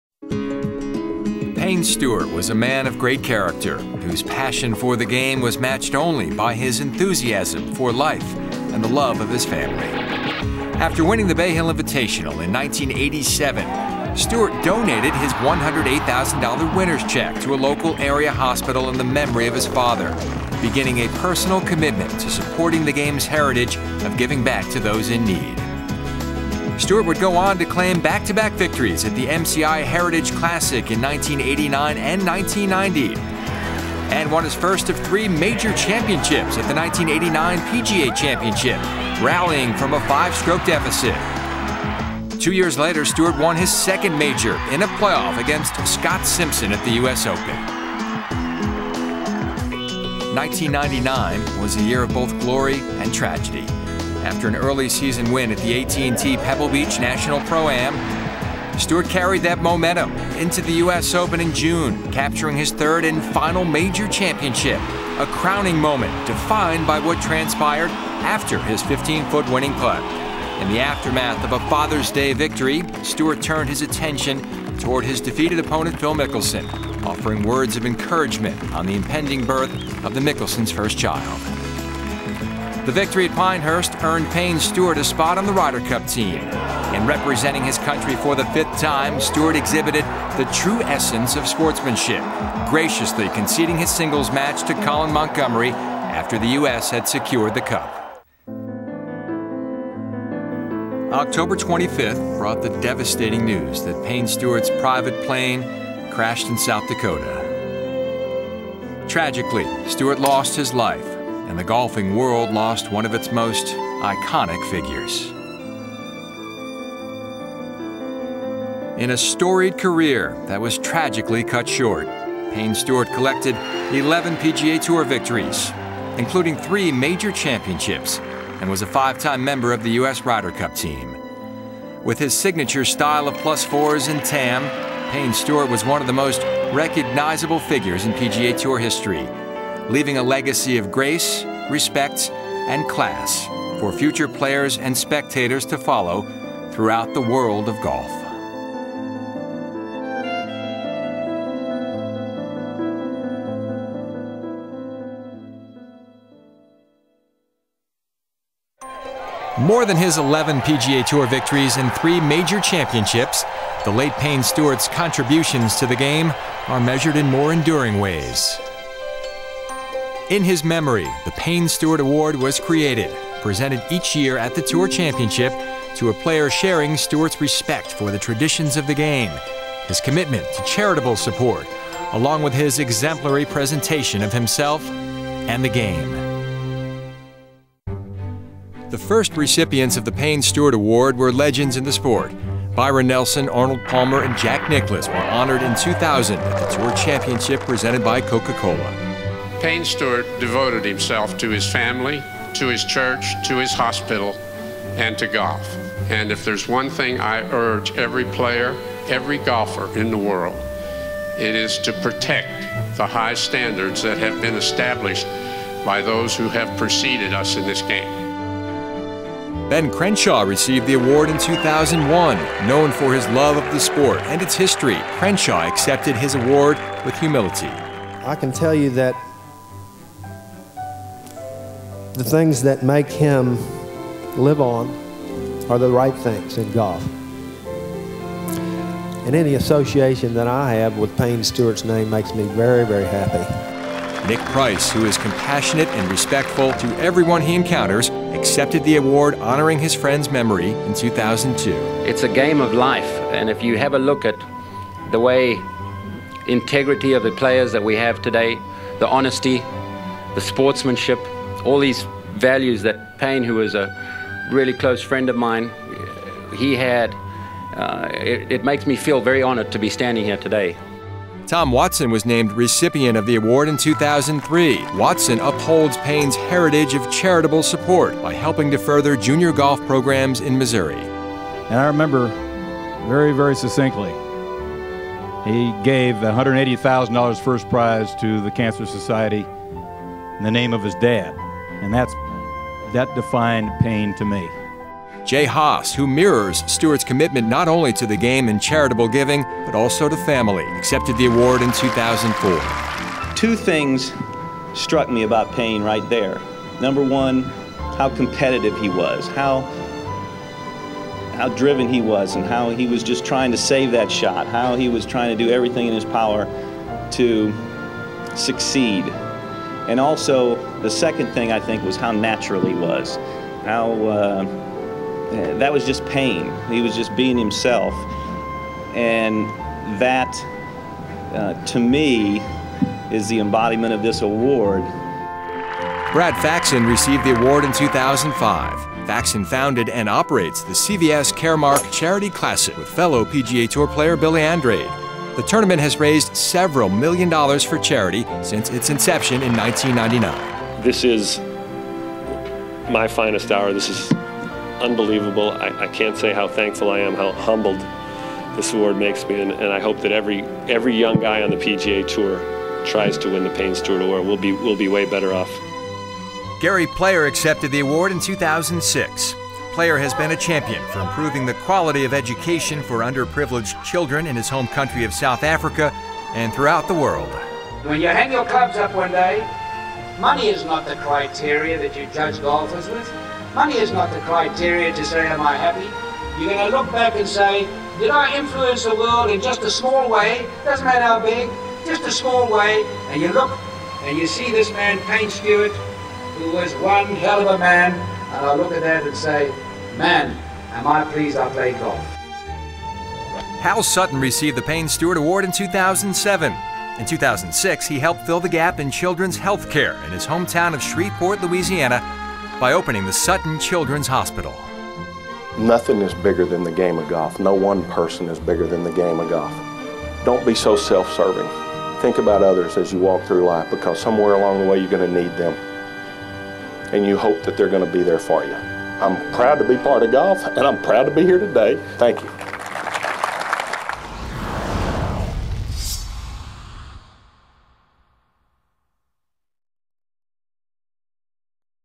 Remembering Payne Stewart with a namesake award
The Payne Stewart award is given to players whose character and passion for the game of golf is matched only by their enthusiasm for life, like Stewart himself. Take a look at Payne's memorable career and the past recipients of this prestigious award. .